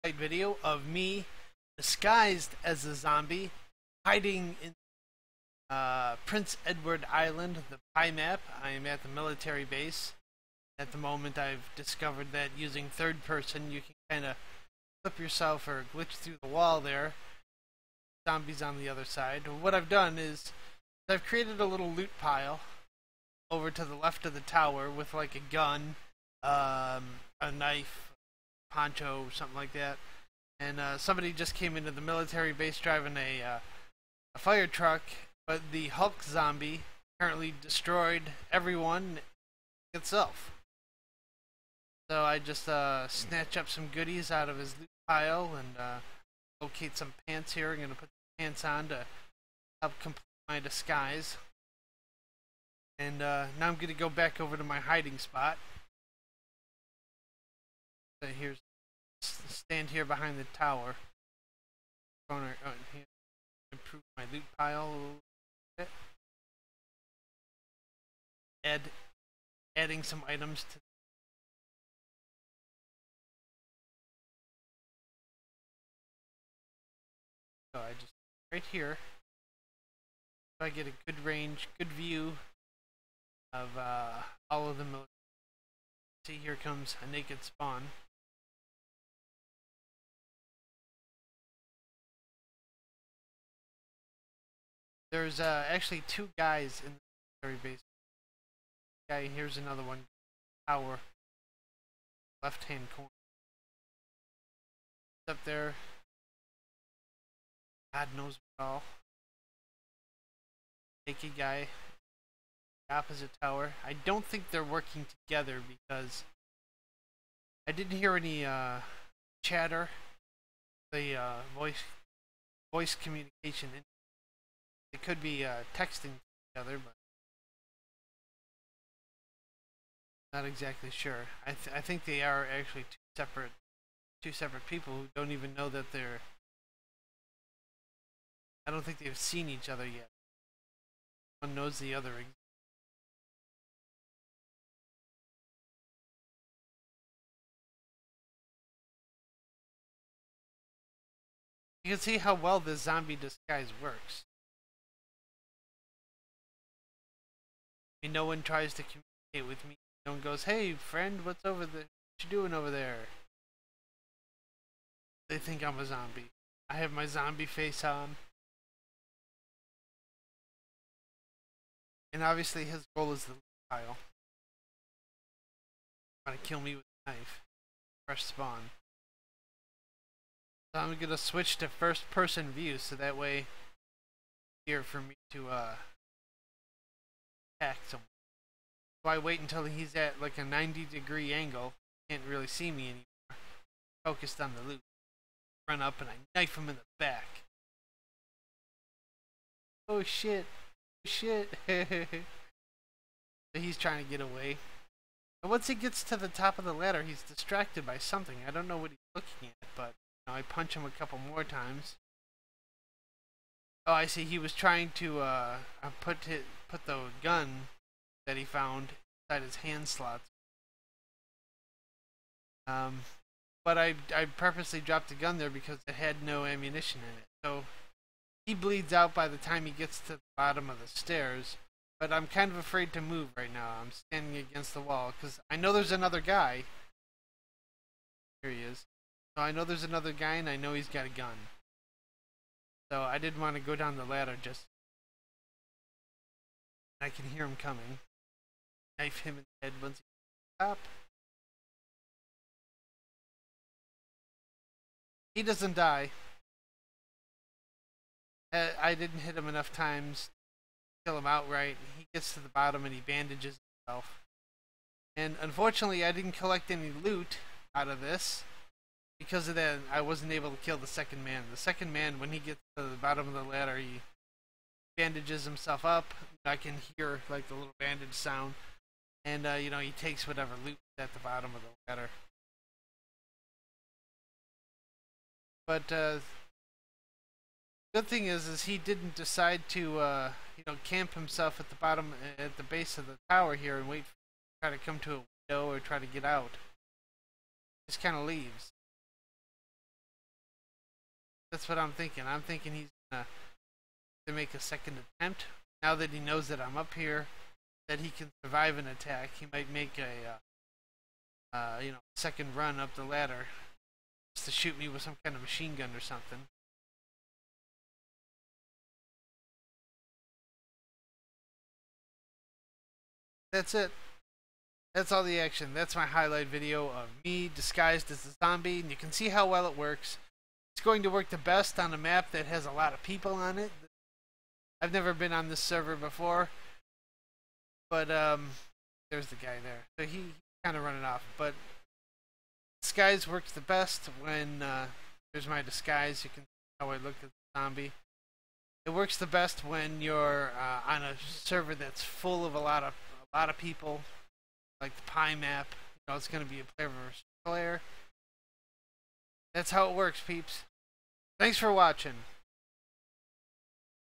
Zombie Disguise
This is me, using a zombie disguise, hiding in one corner of the military base in PEI. I knifed one guy and used his loot pile as a bait to attack another player. Second man bleeds out, dies at bottom of ladder. Third comes up the tower to where I am. I knife him 4 times and he escapes.